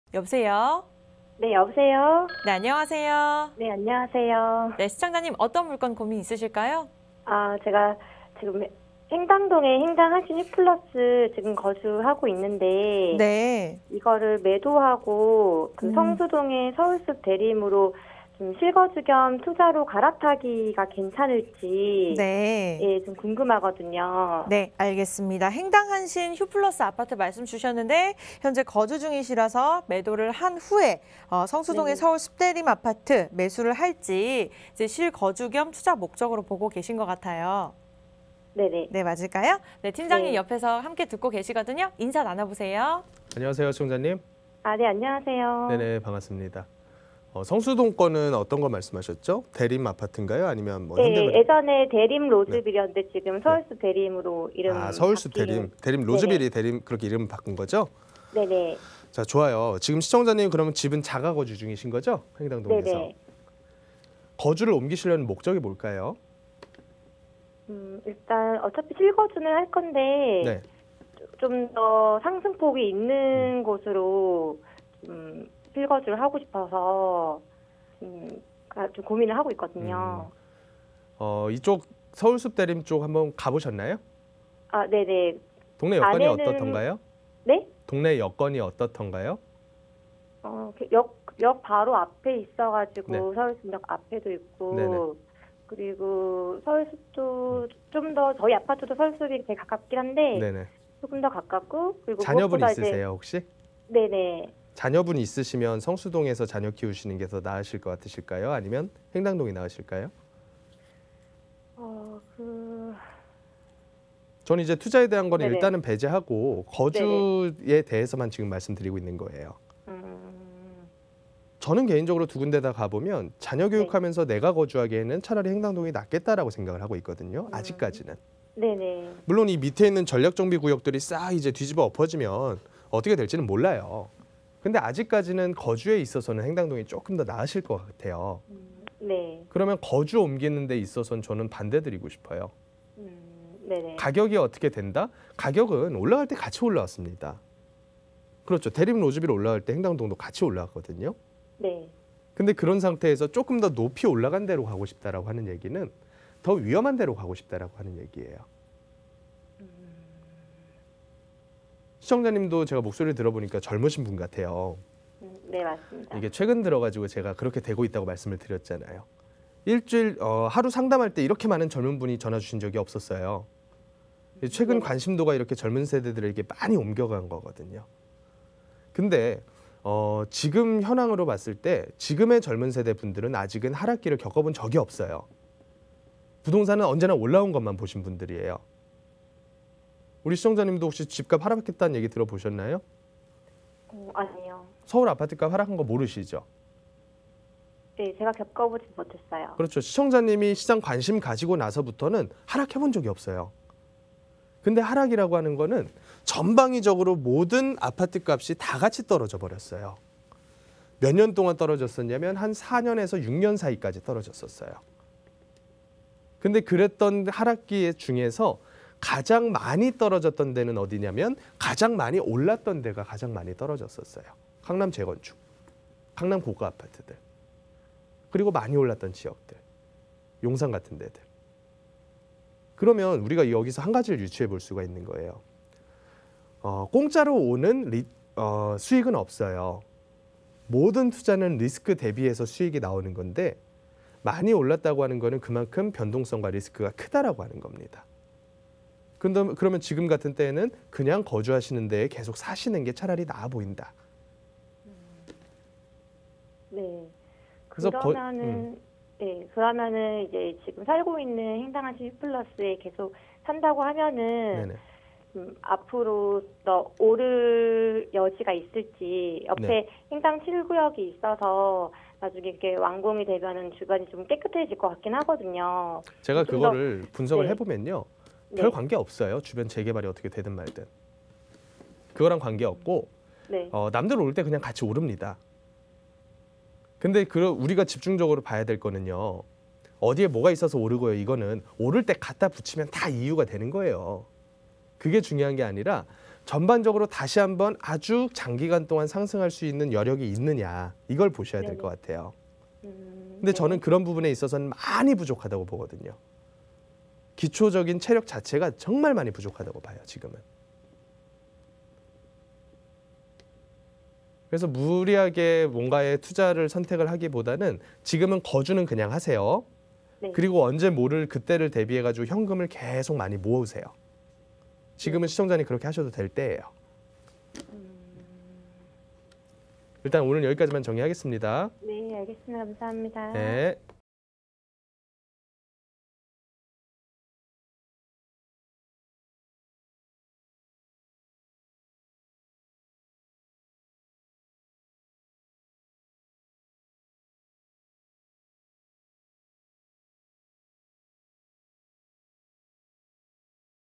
신고가 갈아치우는 행당한신, 가격 상승 여력은?!
- 현 거주지 보다 상승의 폭이 있는 매물로 이주를 고민중인 시청자
- 가격의 상승은 오를때 같이 올랐던 두 매물
- 거주를 옮기는 이주는 반대 보유 매물의 더 거주를 추천
- 김남현팀장은 시청자가 더 위험한 매물로 갈아탄다고 말하는데
왜 더 위험한 매물로 갈아탄다 말했을까?
궁금하시다면 영상에서 확인하세요!